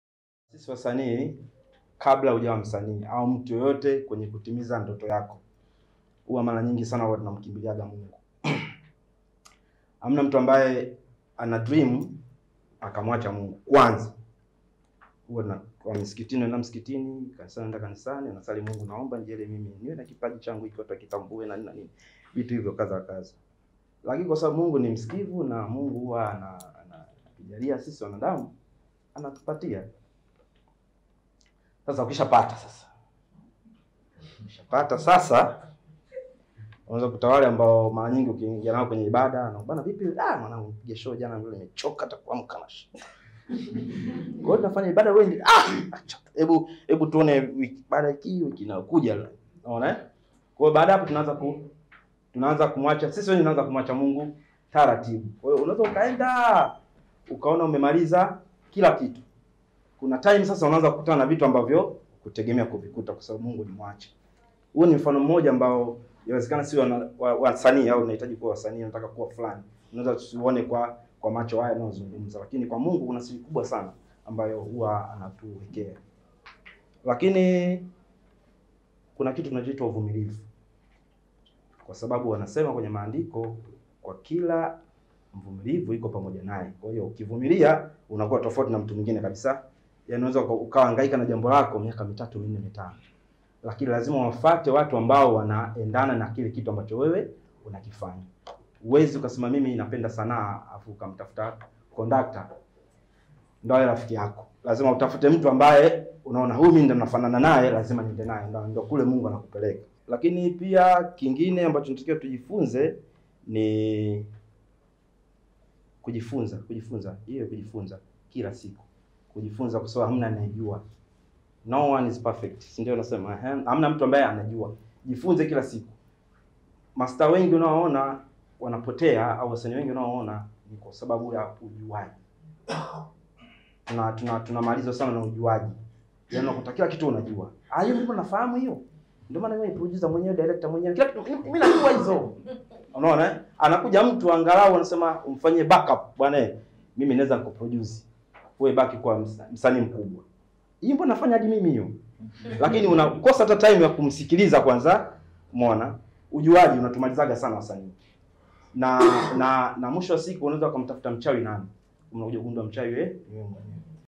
"WACHAWI NI SISI WENYEWE" -JOTI
Awamu ya pili kwa wadau wa Filamu wanaochipukia kuwa maarufu (THE COMING FILM STARS) kwa lengo la kuwaandaa na mabadiliko ya maisha baada ya kuwa maarufu ili wazidi kufanya vizuri katika kazi zao.